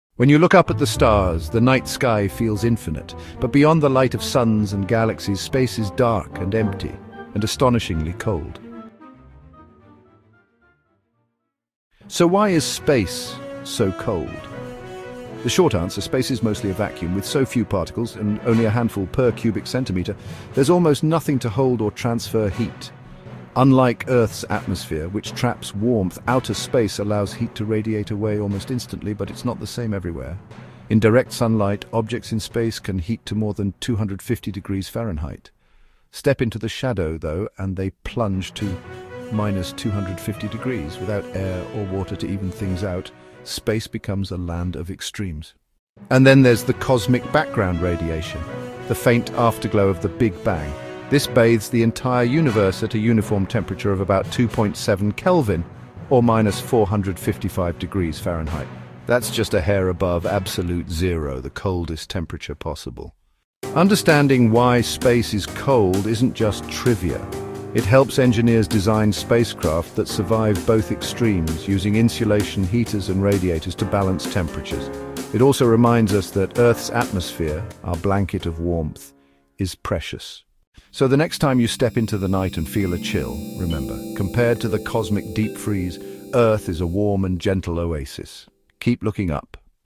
Saturday Space Explainer #4 | Won't Believe How Cold Space Really Is!
Space is both blazing hot and freezing cold - depending on where you stand. ❄️☀️

In sunlight, temperatures soar above 200°F. In shadow, they fall to -250°F. And all across the universe, cosmic background radiation holds everything near 2.7 Kelvin, just above absolute zero.

Keep Looking Up. @D3-DiscoverDeepDreams